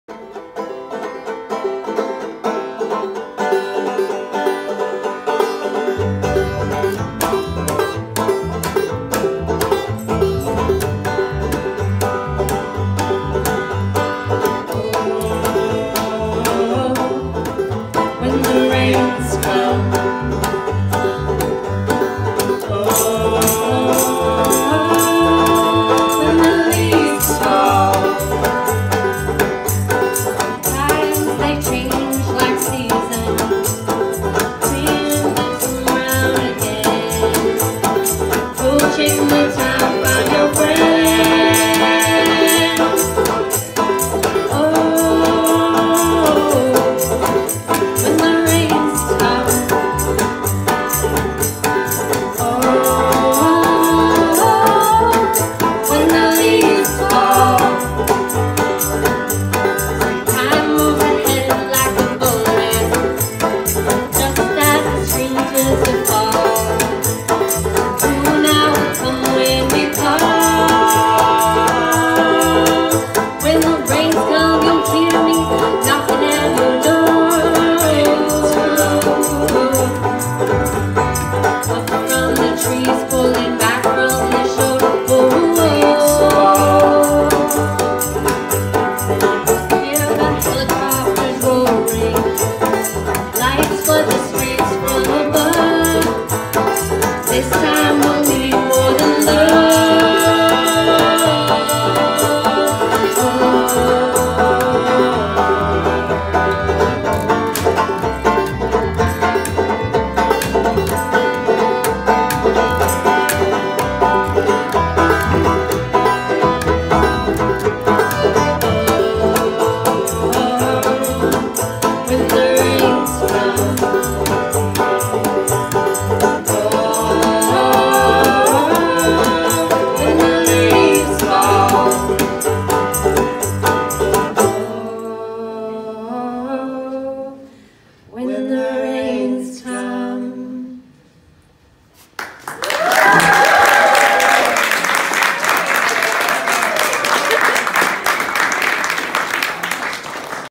Wayfairy Oakland Folk Band 924 Gilman Ghostship Tribute
Wayfairy started with a banjo on the turnpike and has grown into a six piece music project that fills the sonic space between mournful folk and riotous punk. Originally the brainchild of multi-instrumentalist Quiver Watts, Wayfairy has grown into a collective project that blends accordion, banjo, violin, washboard and upright bass with soft vocal harmonies, punctuated by raw wails.